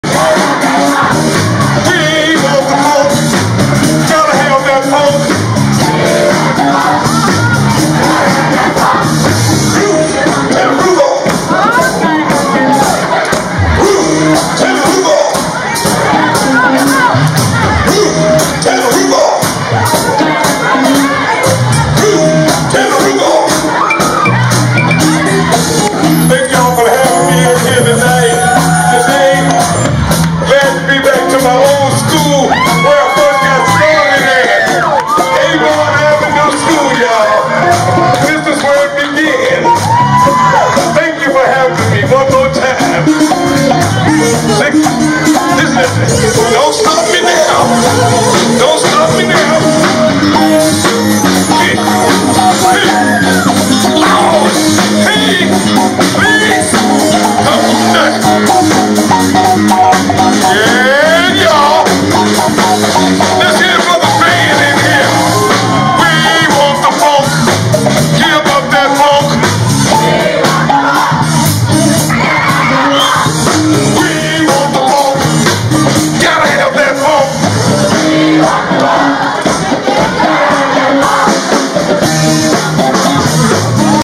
GEORGE CLINTON BAND ROOM NAMING CEREMONY
The band room at Avon Avenue Elementary School in Newark was renamed the George Clinton Band Room in honor of the funk pioneer, who attended school there. He appeared at a ceremony there on March 17, 2022.